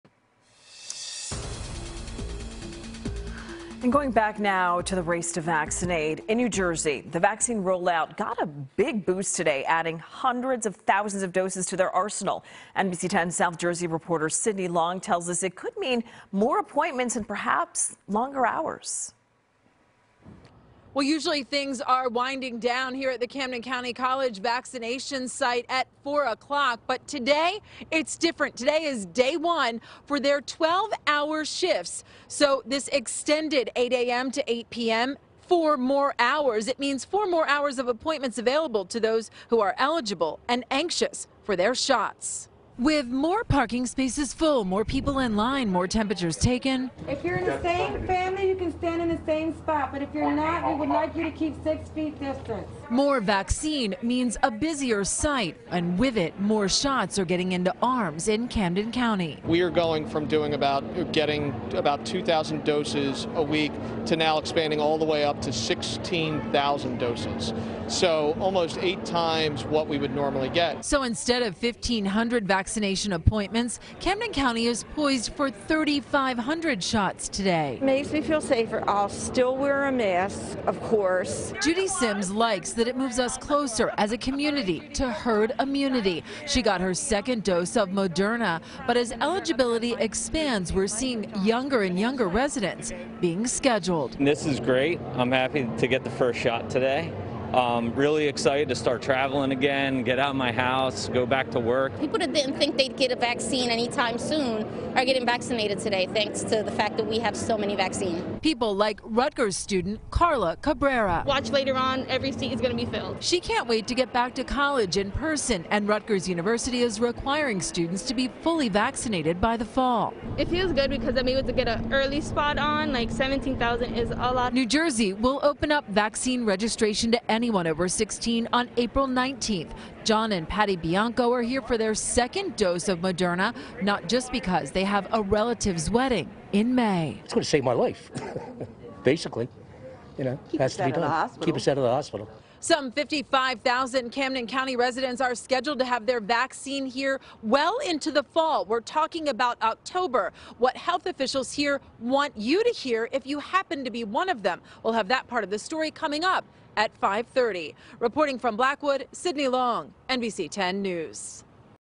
New Jersey Adds Hundreds Of Thousands of Coronavirus Vaccine Doses To Their Supply | NBC10
New Jersey received a big boost to their coronavirus vaccine rollout adding hundreds of thousands of doses to their arsenal. NBC10’s South Jersey bureau reporter Cydney Long tells us this could mean more appointments and possibly more hours.